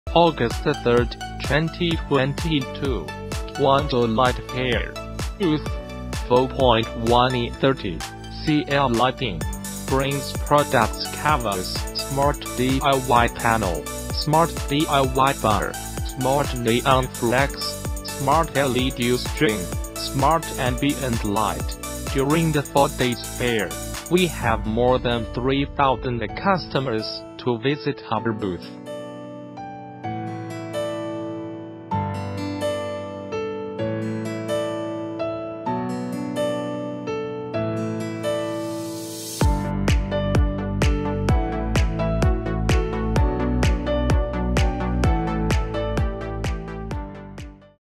CL Lighting 2022 August Guangzhou International Lighting Exhibition (GILE)
The Guangzhou International Lighting Exhibition (GILE) will return to the China Import and Export Fair Complex . As one of the leading fairs for the lighting industry, GILE 2022 saw a significant rise in the number of visitors alongside the concurrent Guangzhou Electrical Building Technology (GEBT). The two fairs attracted 128,202 visitors from 58 countries and regions, which represented an increase of 31% from the previous editions.

The 2022 edition will expand to occupy areas A, B, and the new area D of the China Import and Export Fair Complex in Guangzhou, bringing together over 2,000 exhibitors. Together with the concurrent Guangzhou Electrical Building Technology (GEBT), GILE 2022 will span a total of 21 halls.